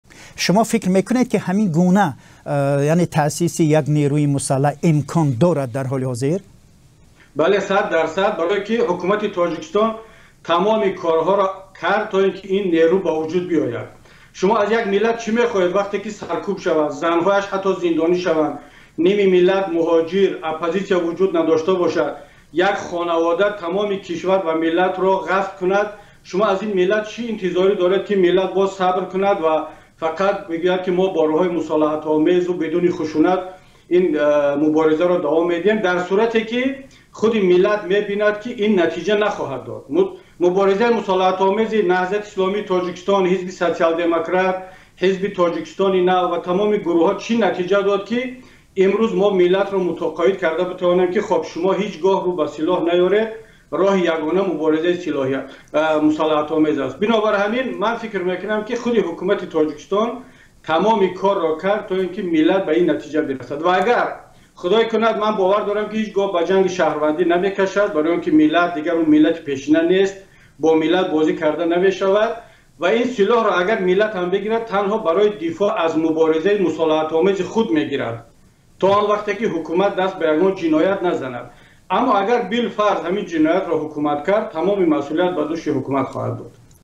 Оё мардум даст ба силох мезанад? | Гулчини сухан
Оё мардум аз зулми Рахмонов даст ба силох мезанад?

бадахшон_хоруғ, шаҳри хоруғ, вазъияти хоруғ, бадахшон, ахбор, tajikistan